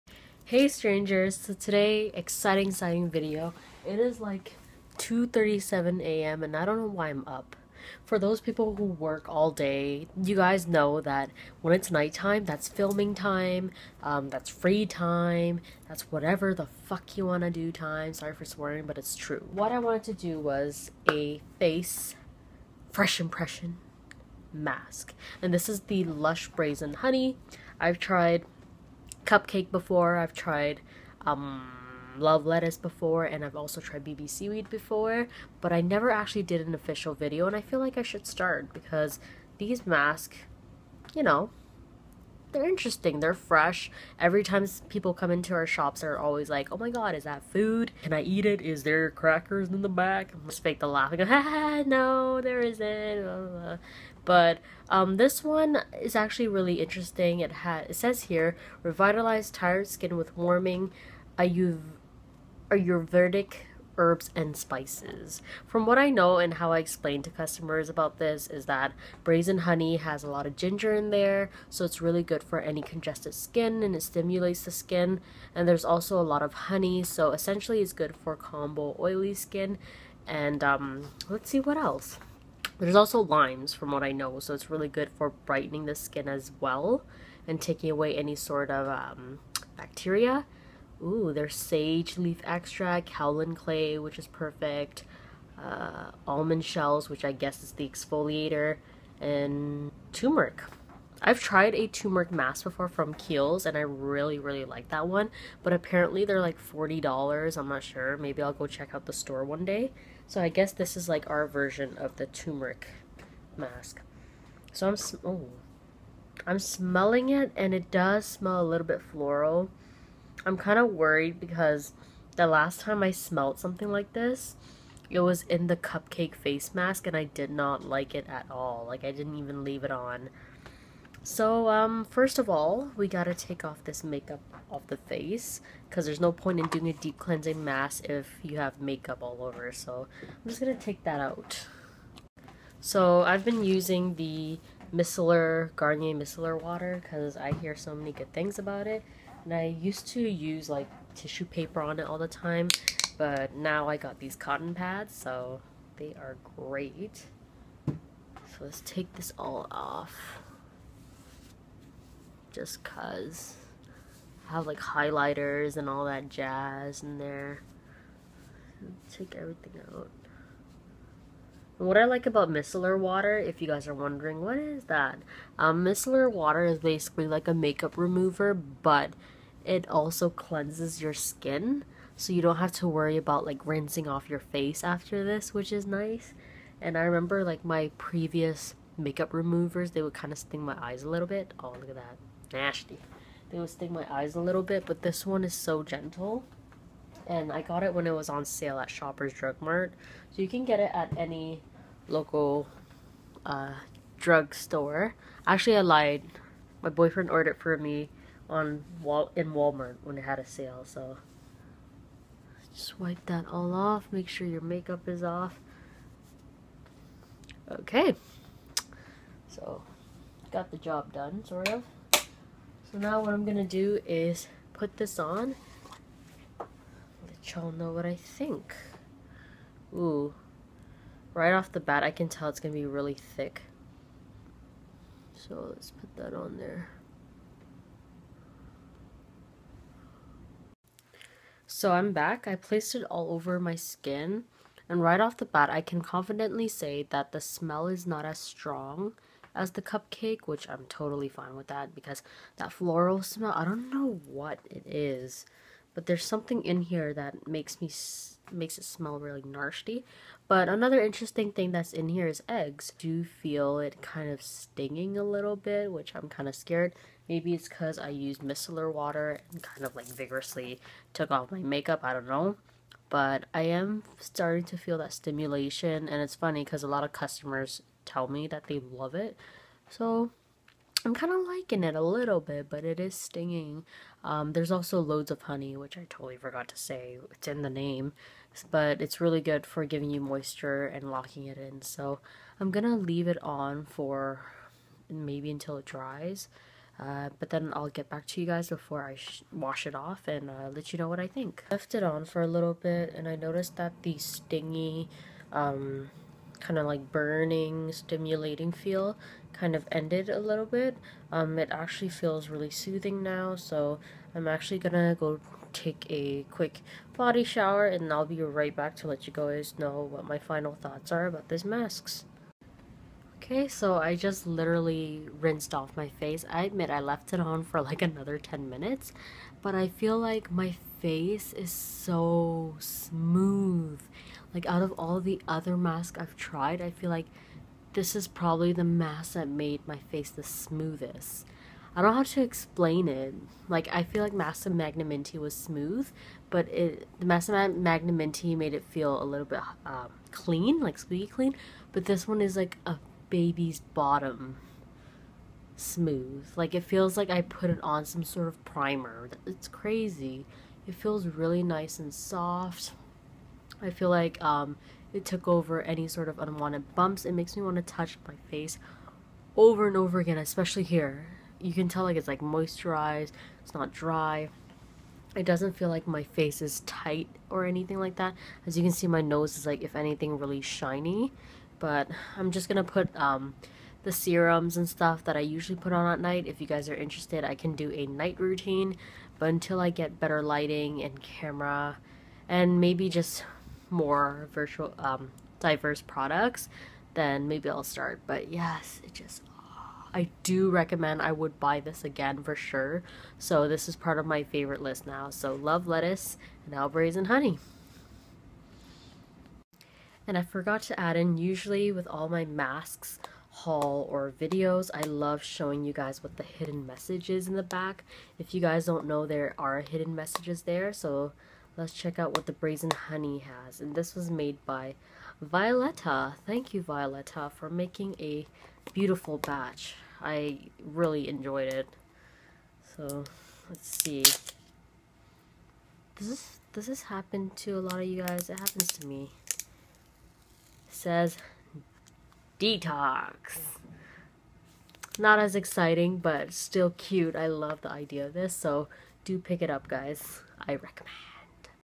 FreshIMPRESSION || brazened honey MASK from LUSH
Hello Strangers,
Here is a new series to my channel. I should have done this for the other masks I tried before as well but maybe I will re-purchase and film it for you guys :) if you like what you see, Please comment, rate and subscribe!!